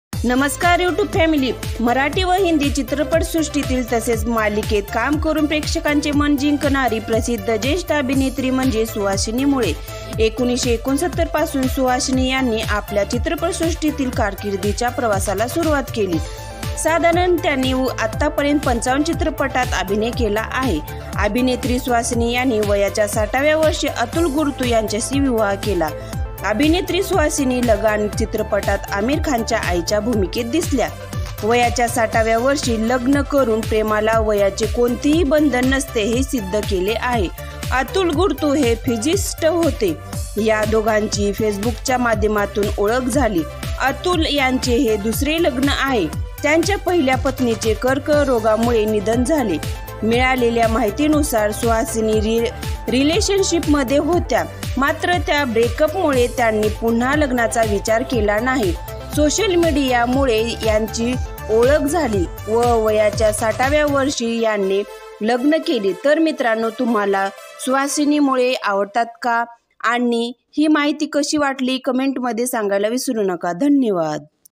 60 यावर्षी प्रेमात पडलेल्या या ज्येष्ठ अभिनेत्रीने लग्न केलं. पहा फोटो
60 यावर्षी प्रेमात पडलेल्या या ज्येष्ठ अभिनेत्रीने लग्न केलं. पहा फोटो